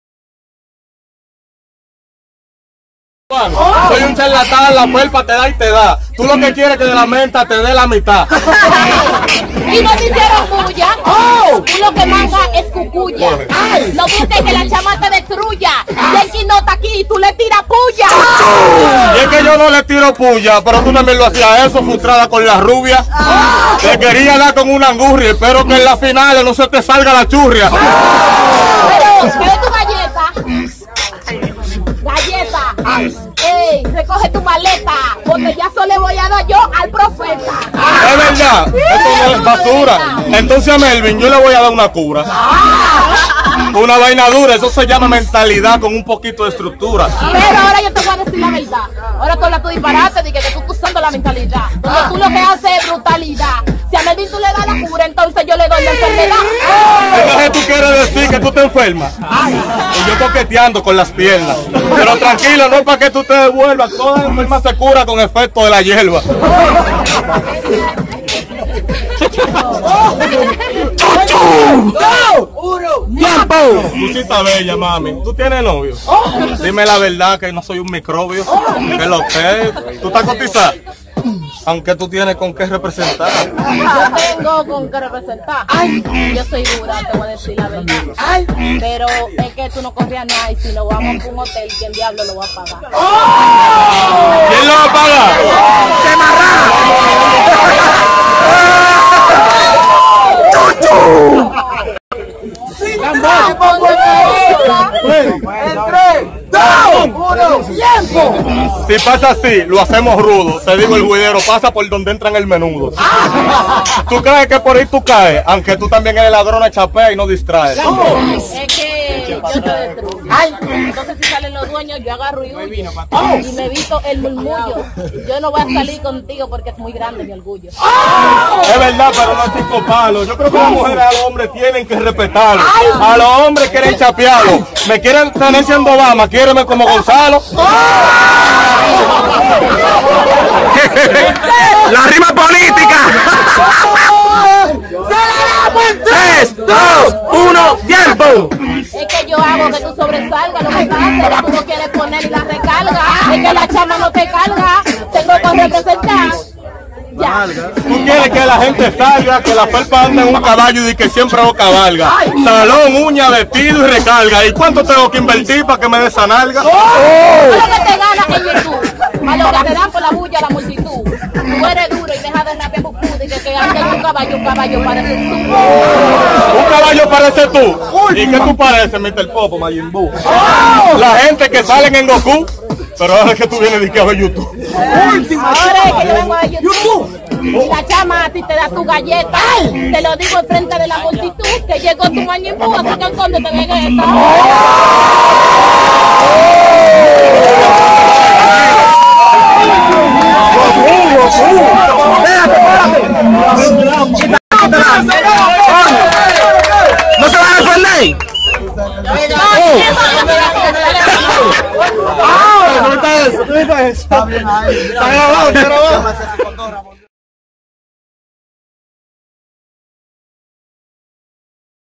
FELPA DIVO VS LA CHAMARD (CENTRO OLÍMPICO) JUNTE FREESTYLE
DEMASIADO DUROS OH DIOS 🔥! VENEZUELA VS DOMINICANA -ADONYS & STHONNY VS R8 & El PROFETA